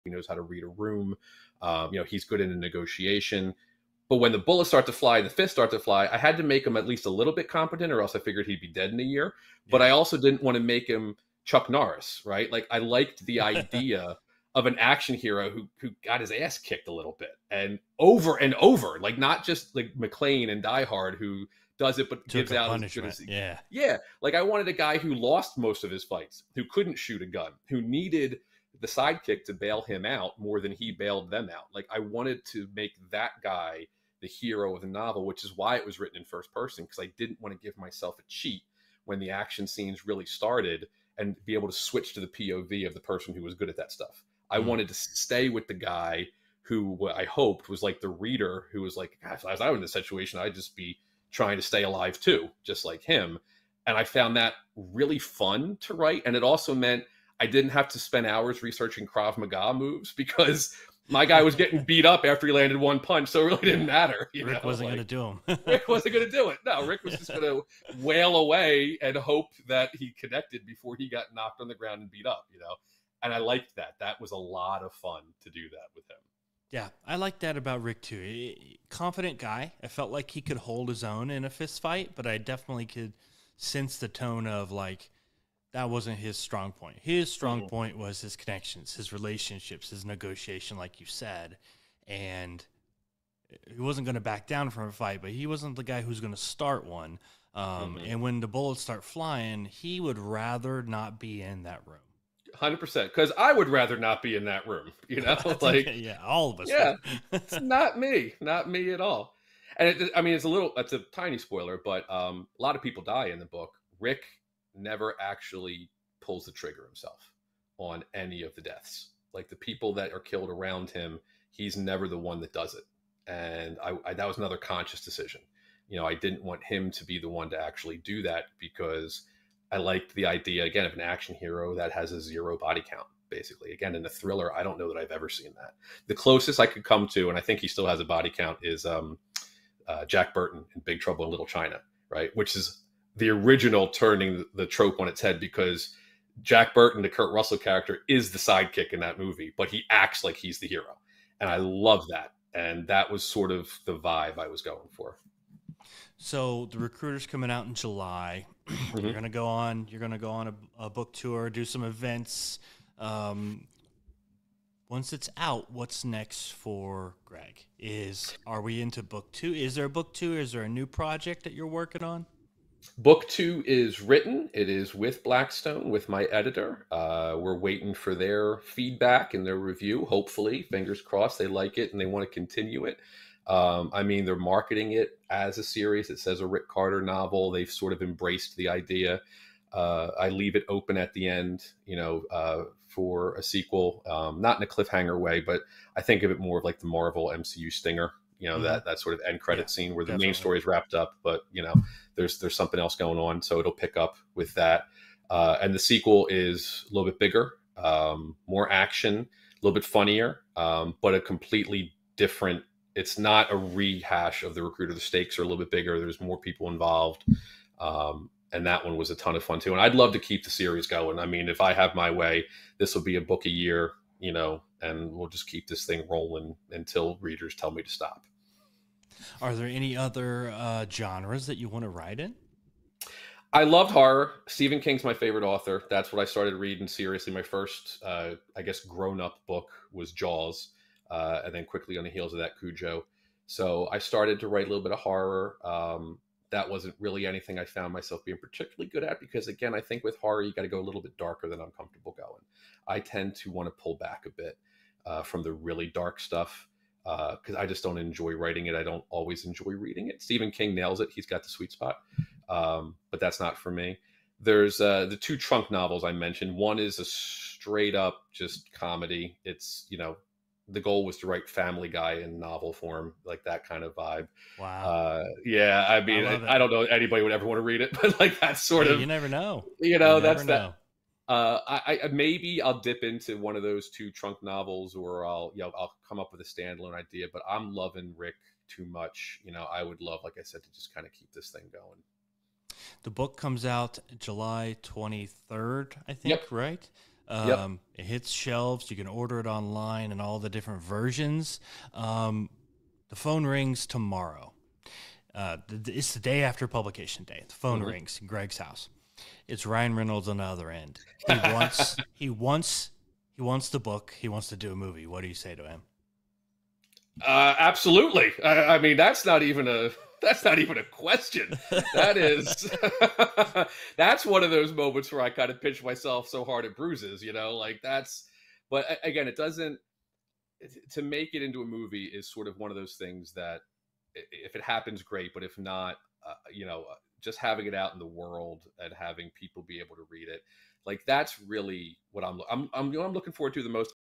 The Accidental Action Hero
Author Gregg Podolski on how he created his character Rick Carter